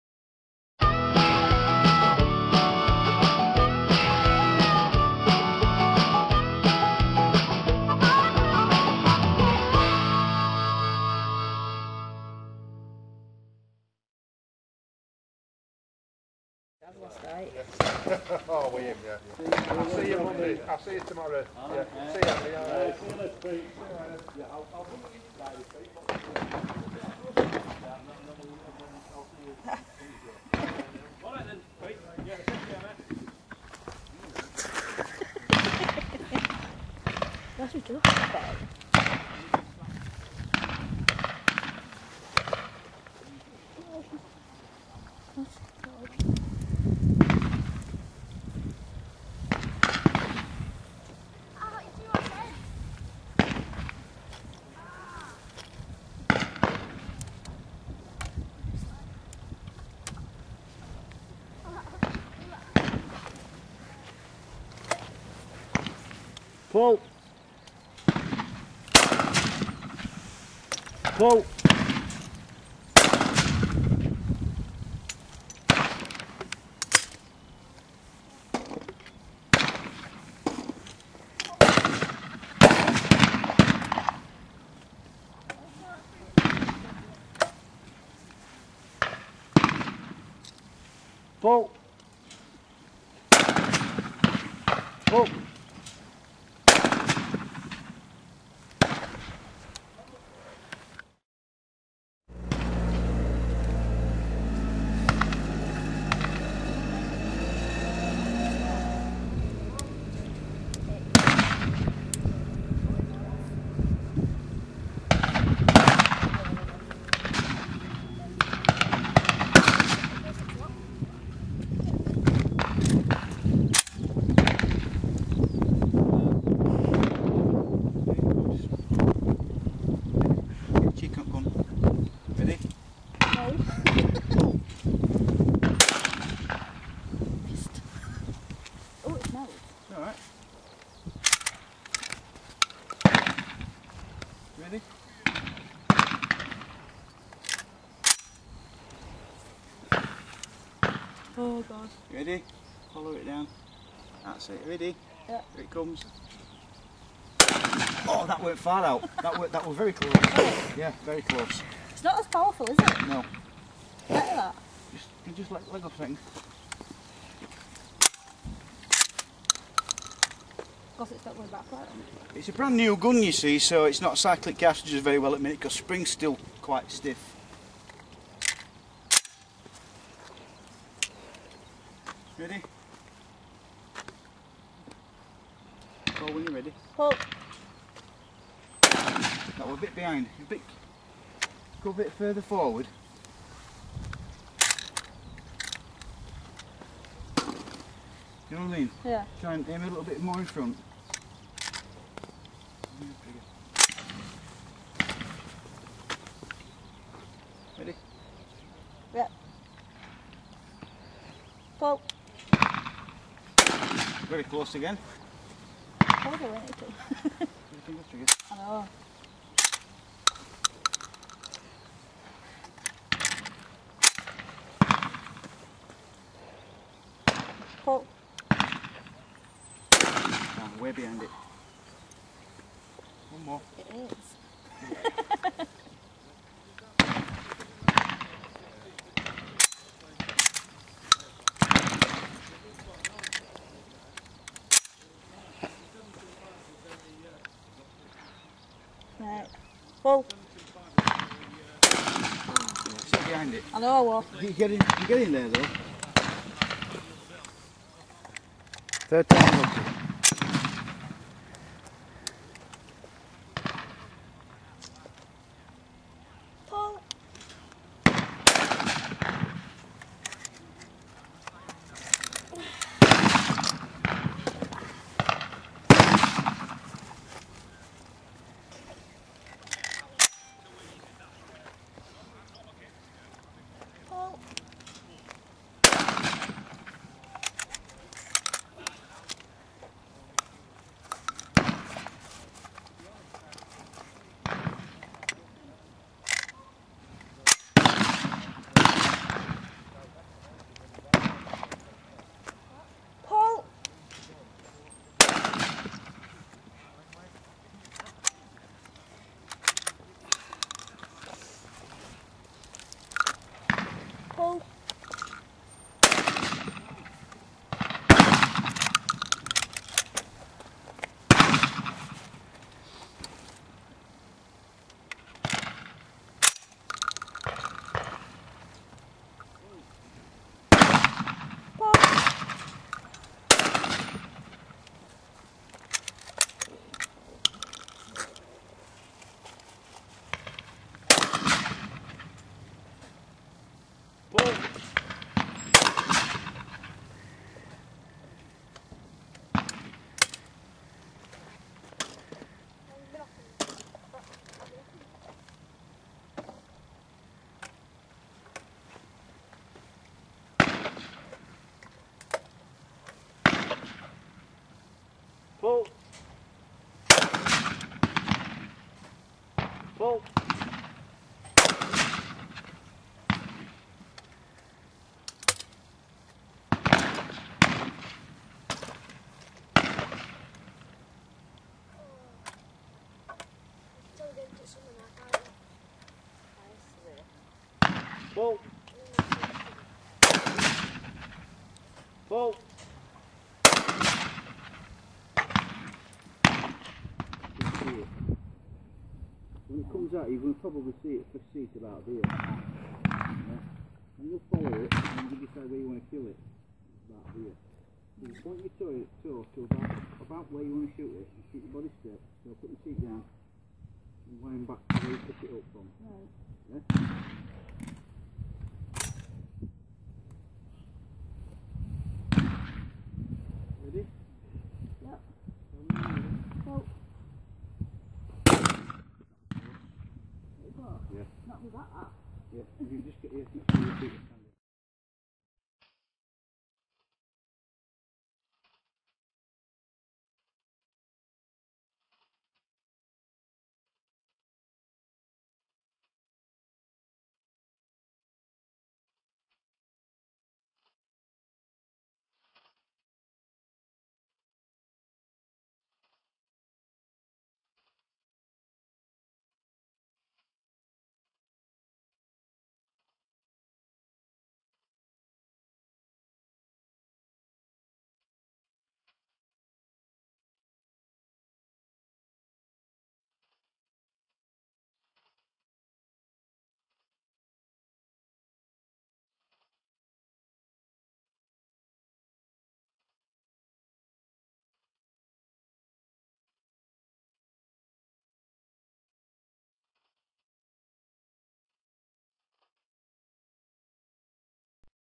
20g fun at the shooting school (please read the description)
Had a great morning with my daughter,son and my good friend Andy.  Lisa was apprehensive but after the first few shots she was hooked, as most people are when they try it ! She still has to get the hang of keeping that stock firmly tucked into the cheek and not looking up to check at the last moment before she fires. The day was meant to be an introduction only so we will work on that later. The gun was my new 20g Hatsan Escort Magnum which had a few issues with cycling. 24 gram cartridges and it would occasionally "bite" the case with the ejector leaving a deep dent that left nothing for it to grab. The last shot of the day left me having to push the empty case out from the other end with the cleaning rod! :-(